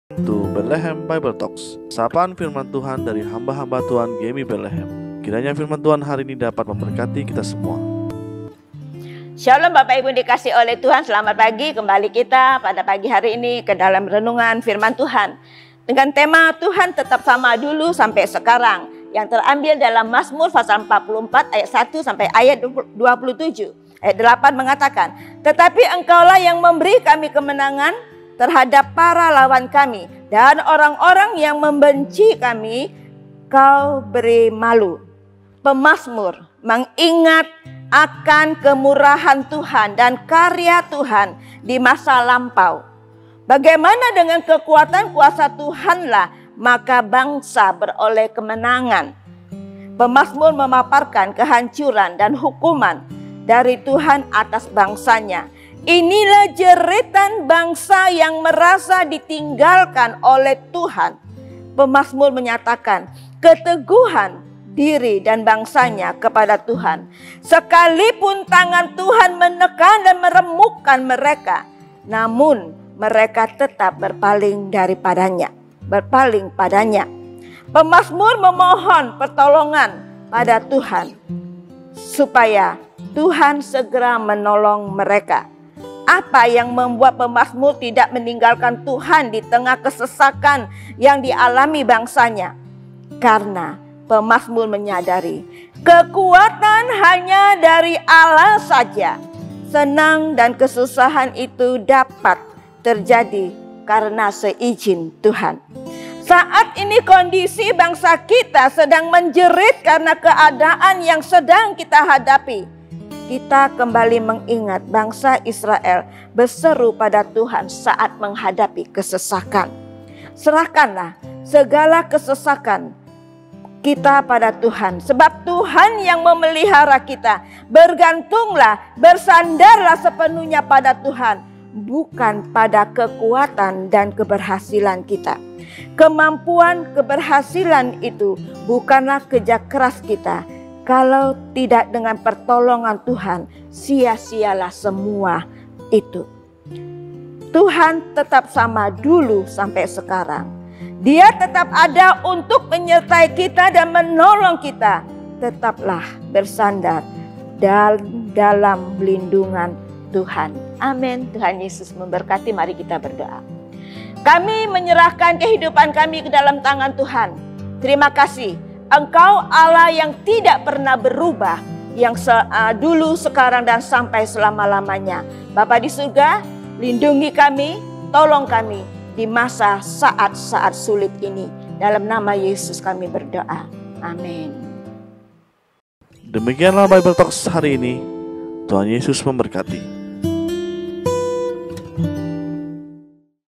TUHAN TETAP SAMA DULU SAMPAI SEKARANG | Bethlehem Bible Talk | Selasa, 27 Juli 2021
Welcome to Bethlehem Bible Talk, Sebuah program sapaan Firman Tuhan dari Hamba-hamba Tuhan GMI Bethlehem setiap hari Senin-Jumat. Kiranya Firman Tuhan hari ini dapat memberkati kita semua.

Mazmur 44:1-27
44:8 Tetapi Engkaulah yang memberi kami kemenangan terhadap para lawan Kami, dan orang orang yang membeci kami Kauberi malu.

Setiap orang yang mendengar perkataan-Ku ini dan melakukannya, ia sama dengan orang yang bijaksana, yang mendirikan rumahnya di atas batu.
Matius 7:24